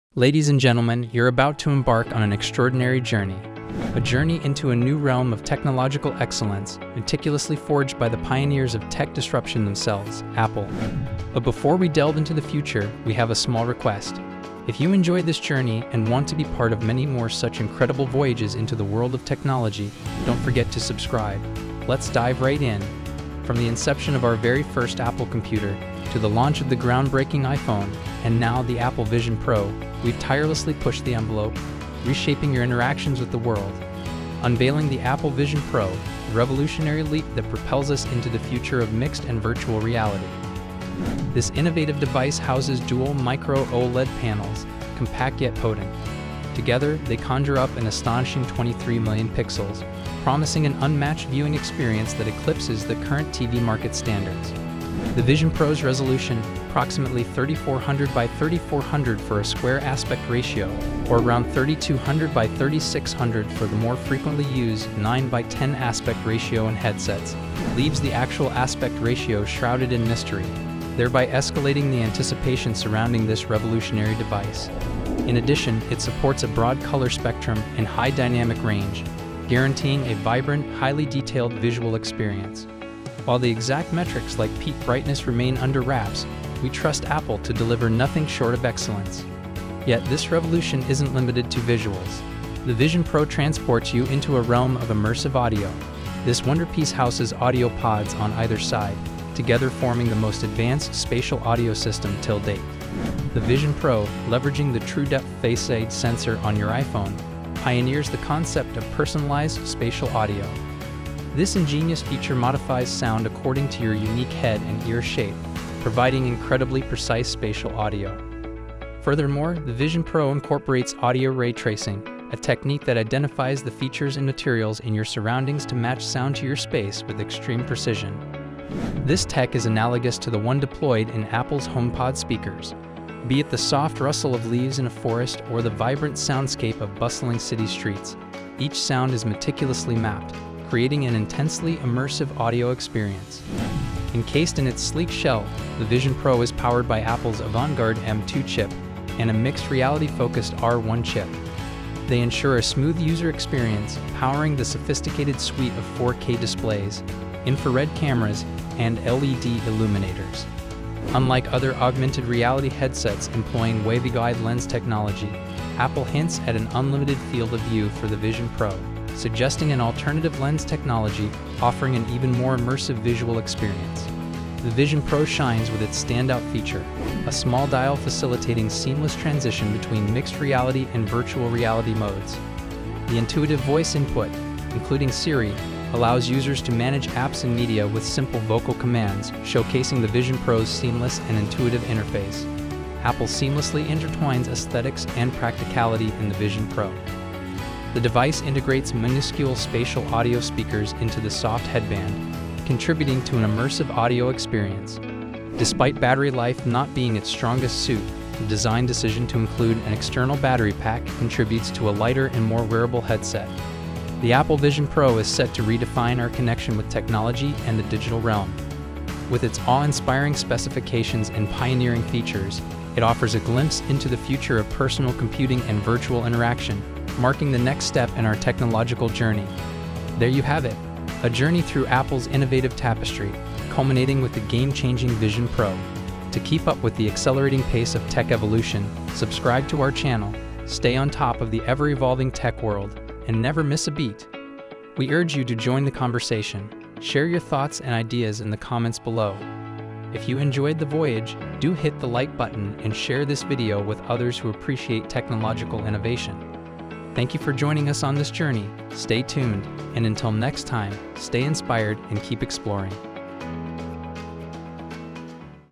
Apple Vision Pro | Introducing the all new Vision Pro
Welcome to an extraordinary journey into the realm of technological excellence with Apple. Join us as we unveil the Apple Vision Pro, a revolutionary leap into the future of mixed and virtual reality. With dual micro-OLED panels and a staggering 23 million pixels, this device promises an unmatched viewing experience. Experience vibrant visuals with a broad color spectrum and high dynamic range. The Apple Vision Pro also immerses you in personalized spatial audio, adapting sound to your unique head and ear shape. Explore the world around you with Audio Ray Tracing, matching sound to your surroundings with precision. Powered by Apple's advanced M2 chip and a mixed-reality-focused R1 chip, this sleek headset offers a smooth user experience. Discover an unlimited field of view and seamlessly transition between mixed-reality and virtual-reality modes with a small dial. With intuitive voice input and a lightweight design, the Vision Pro merges aesthetics and practicality. Prepare to redefine your connection with technology and the digital realm. Thank you for joining us on this extraordinary journey. Stay inspired and keep exploring.